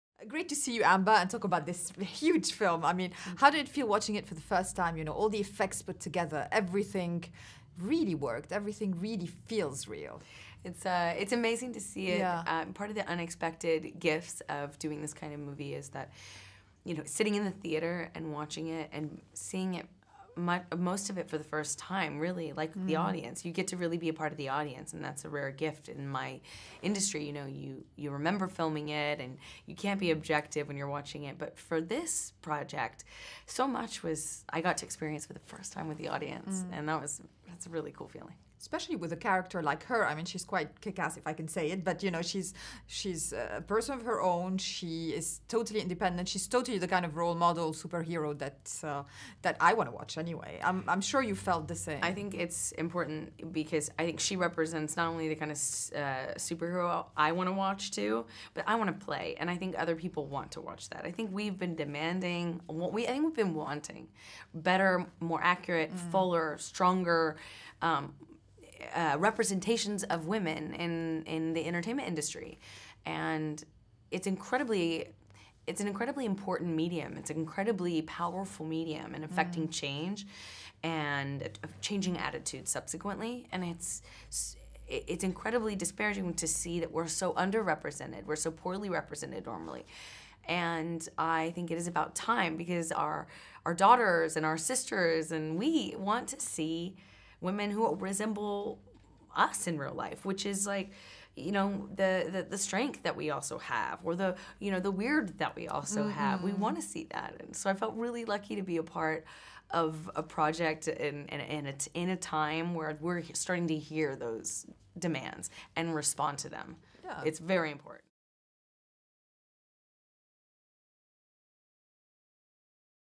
Amber Heard at the Aquaman press junket in New York
Amber speaks about seeing Aquaman in theaters for the first time with the audience.